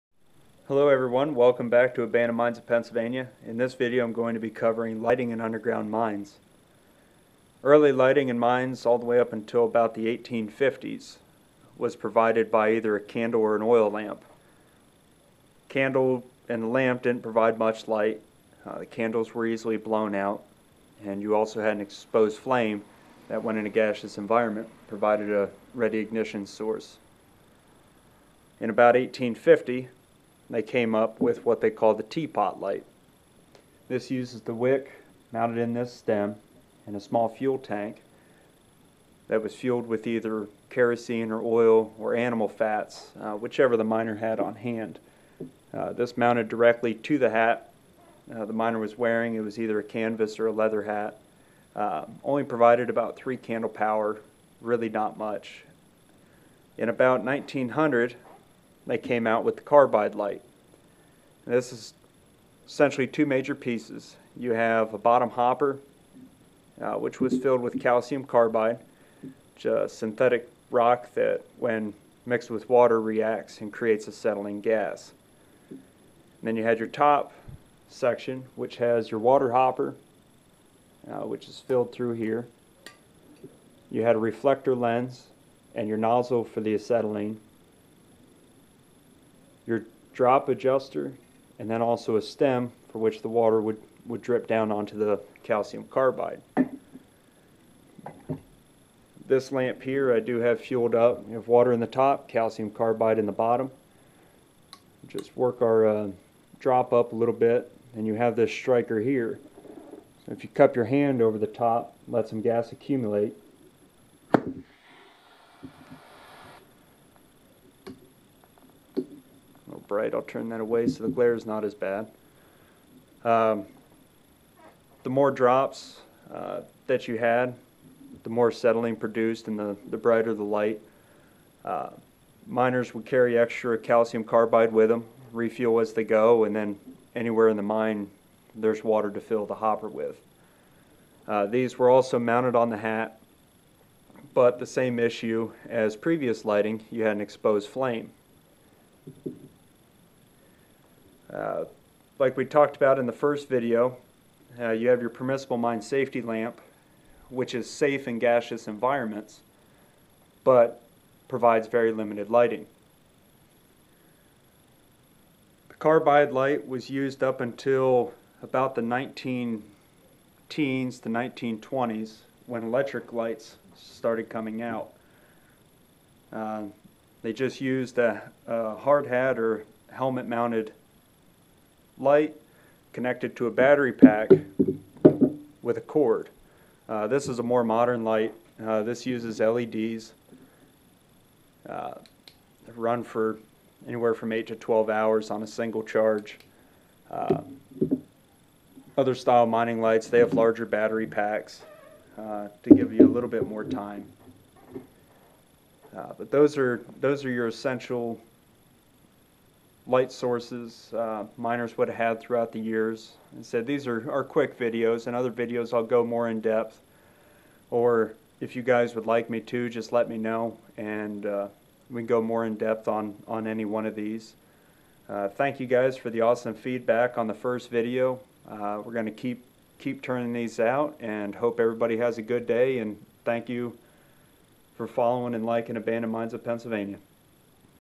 Lighting in Underground Mines
A video briefly describing the different types of lights miners used throughout time. From candles to oil lamps, carbide to LED.